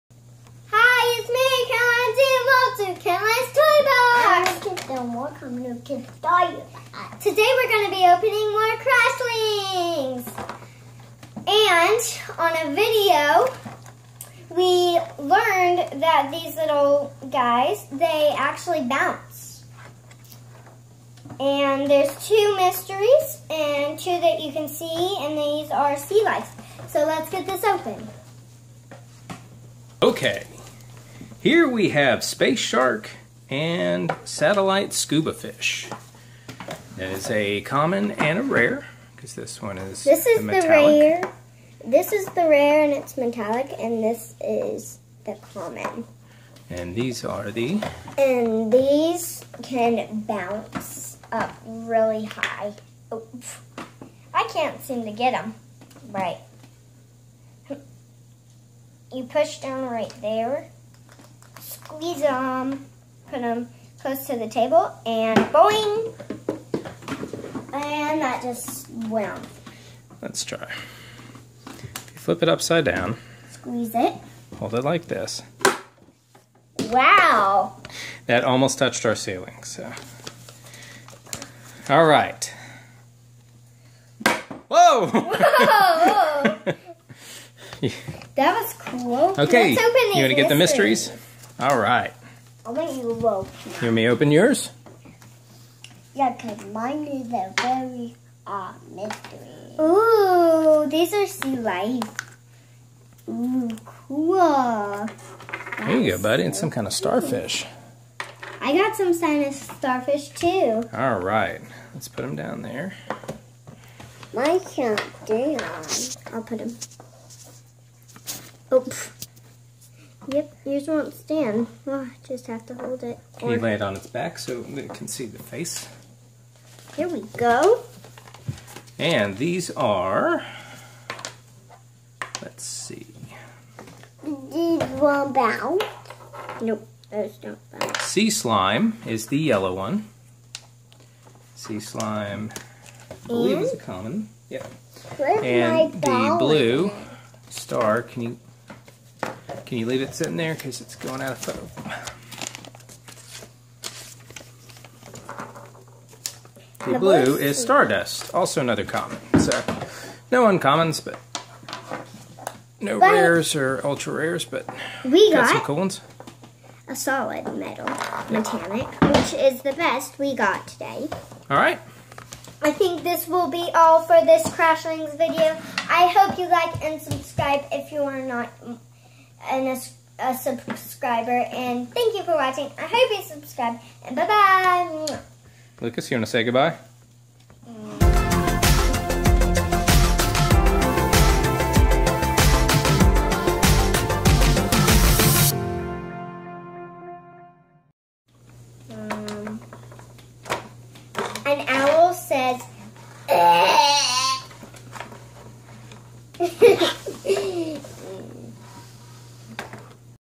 Crashlings Sea Life 4 Pack Opening
Lucas gets some camera time as he opens a Crashlings 4 Pack.  This is from the Sea Life series.
Check out all of our other toy openings, including a variety of Pokemon openings; booster packs, promo boxes, blister packs, tins, and maybe even a few booster boxes.